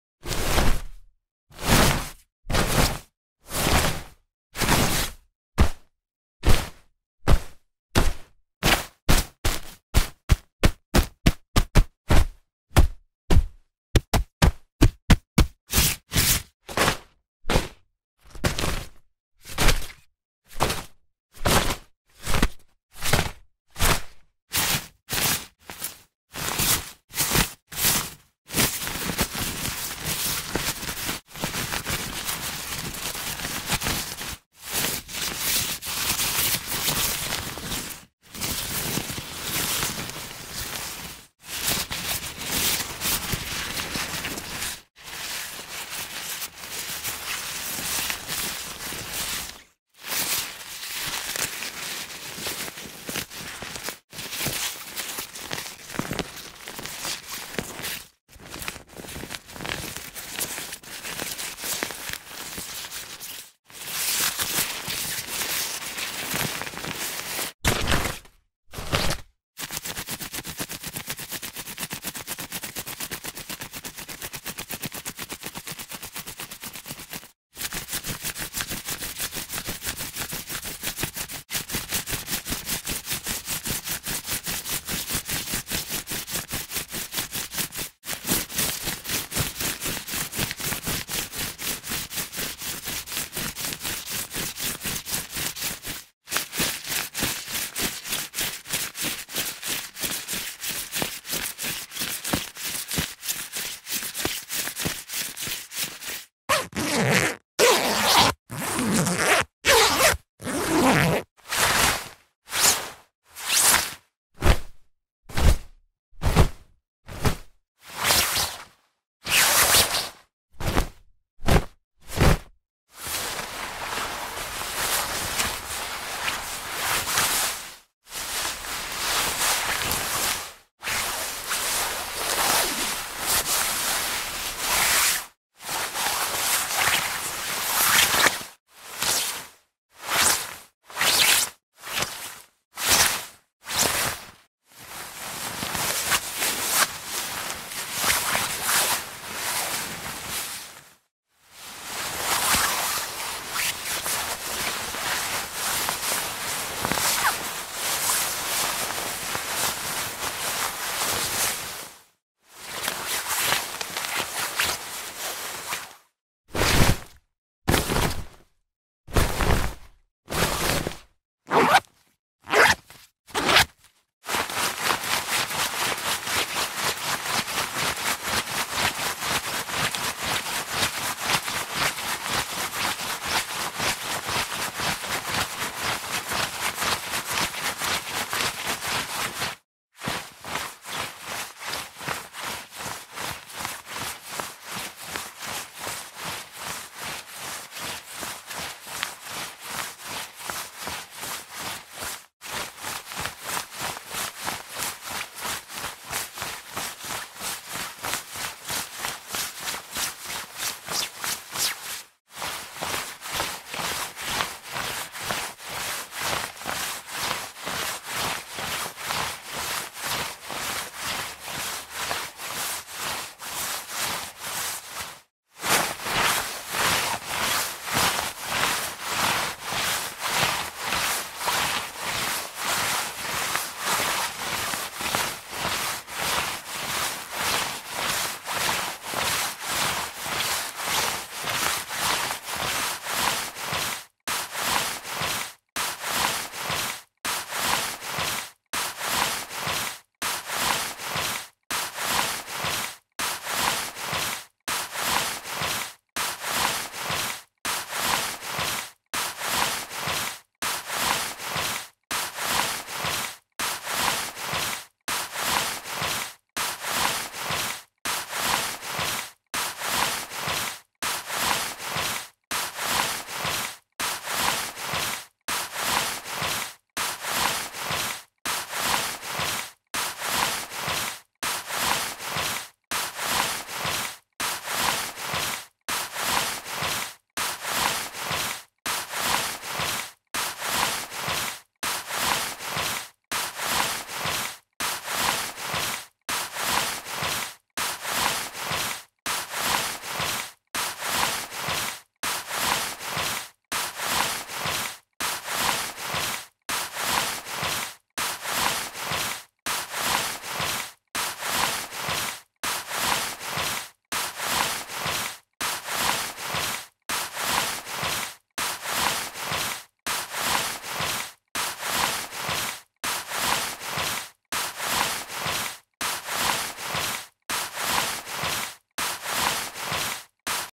Raincoat Movements | 104 FREE raincoat movement sound effects
104 FREE raincoat movement sound effects, perfect what you need various coat or jacket sound effects that provide ultimate flexibility. The sounds included in this pack are recorded and edited by professionals and include various movements and actions, such as pickups, set downs and drops, handling, walking and running movements, zippers, sleeves, and more.

I have a hobby of collecting, will continue to include a variety of sound effects.If you like, you can continue to follow this channel.😊😊😊